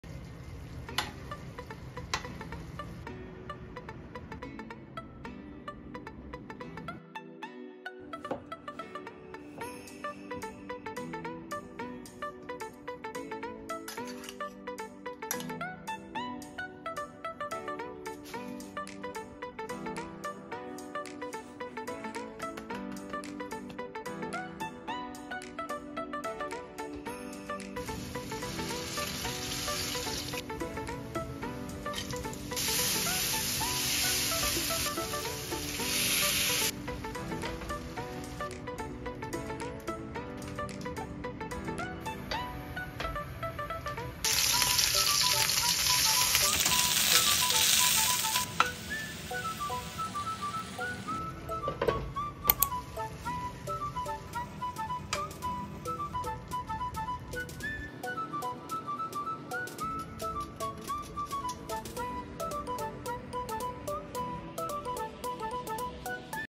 スーパーで買った鶏肉が驚くほど美味しくなります【鶏もも肉のトマト煮】
スーパーで買った鶏もも肉が驚くほど美味しくなります『鶏もも肉のトマト煮』

◾️料理の質問、感想などコメントしてもらえたら嬉しいです☺️

【材料】
鶏もも肉　　　　　　　300g
トマト　　　　　　　　大1個

塩　　　　　　　　　　少々
コショー　　　　　　　少々

水　　　　　　　　　　100ml

パルミジャーノチーズ　少々
（粉チーズで代用できます
イタリアンパセリ　　　少々

◾️味見をして薄ければ塩で調整してください

◾️水の代わりにチキンブイヨンスープを使うとさらに美味しくなります